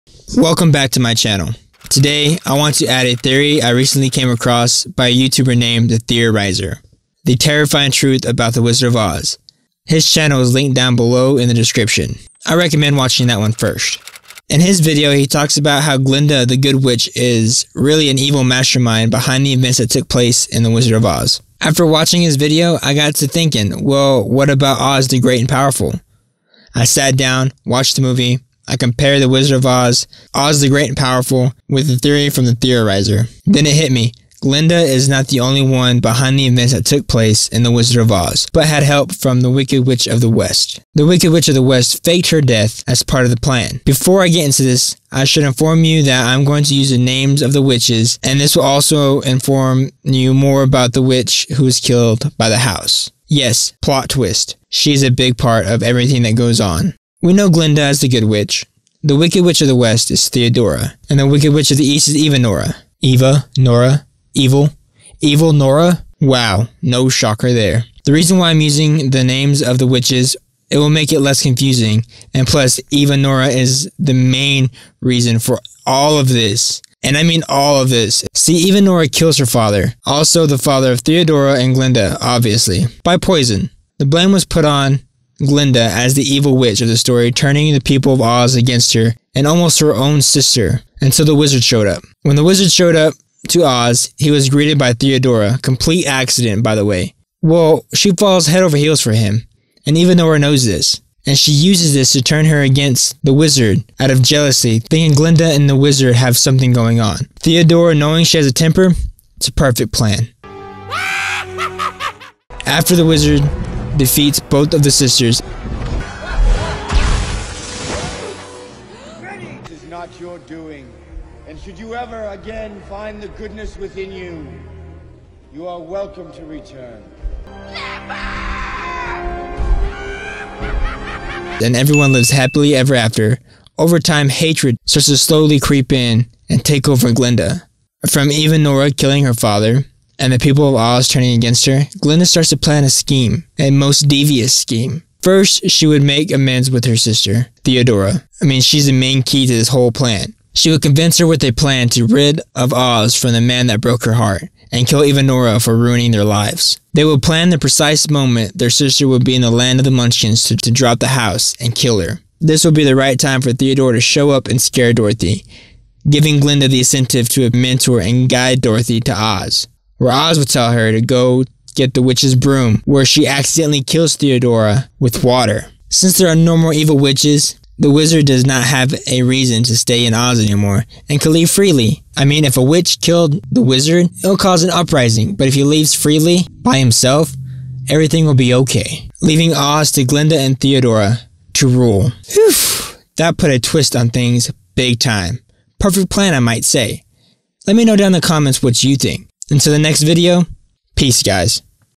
THE DARK SECRETS OF THE LAND OF OZ [THEORY]
WELCOME BACK

 So the other day I came across a theory about the terrible truth of The Wizard of Oz and I'm not going to lie it got me think about the prequal The Great and Powerful OZ and I stumble upon a theory ABOUT THE DARK SECRETS OF THE LAND OF OZ I hope you enjoy. His channel is linked below right here.